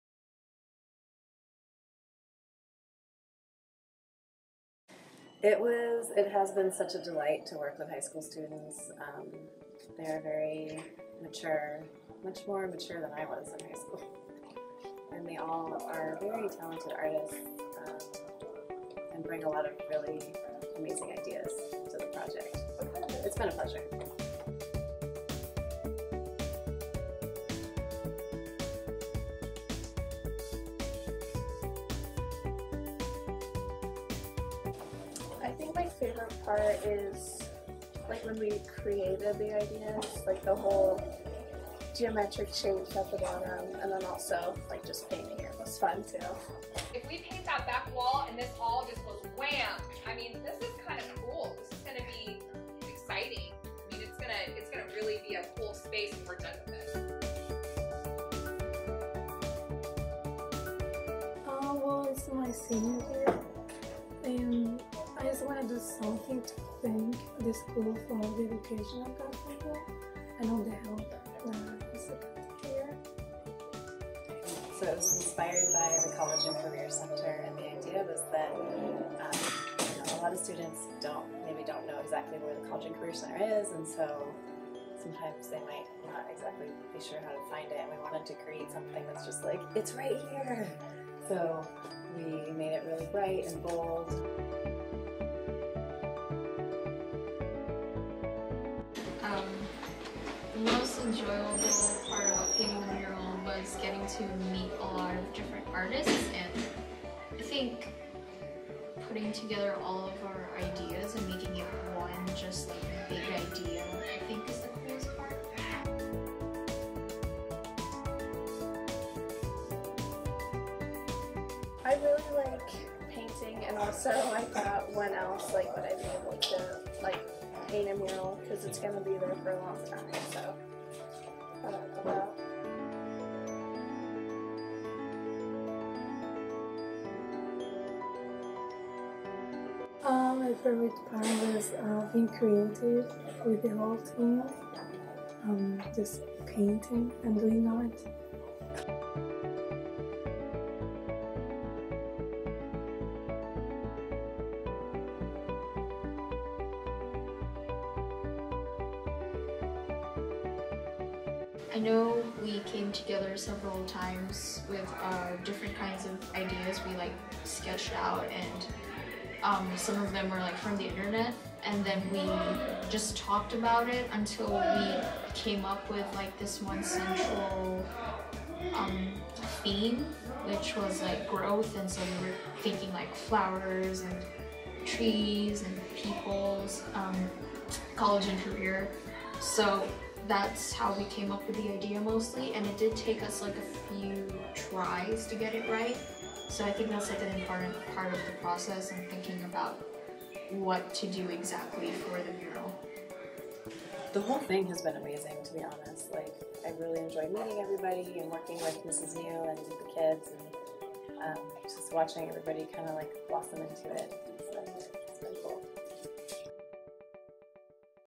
College & Career Center Mural Documentary | WHS
Documentary following the creation of the mural painted at Willamette High School's College and Career Center.

This video was produced by Multimedia Arts students at Willamette High School.